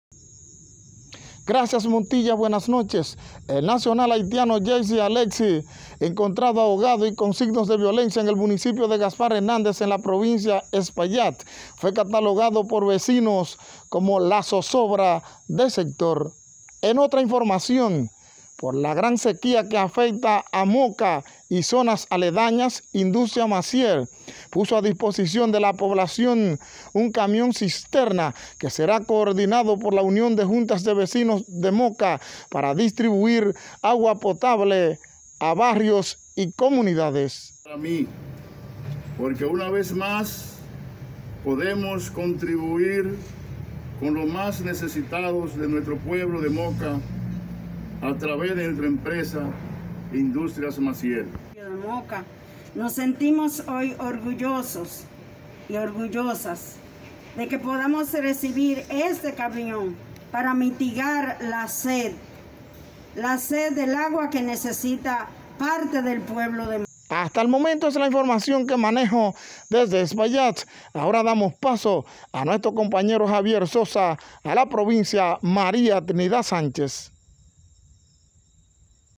COBERTURA/ ESPAILLAT/ARROYO
Lunes 27 de julio 2020

 📌_TEMAS_ 📍

 1⃣NACIONAL HAITIANO  ENCONTRADO AHOGADO CON SIGNOS DE VIOLENCIA EN GASPAR HERNÀNDEZ PROVINCIA ESPAILLAT, JEISI ALEXIS TENÍA EN ZOZOBRA A RESIDENTES DE VERAGUA SEGÚN VECINOS. 🚔

2⃣POR ESCASEZ DE AGUA POTABLE EN MOCA INDUSTRIA MACIER PONE A DISPOSICIÓN DE LA POBLACIÓN  CAMÍÓN SISTERNA PARA DOTAR DE AGUA  A TRAVÉS DE LA UNIÓN DE JUNTAS DE VECINOS A BARRIOS Y COMUNIDADES 💧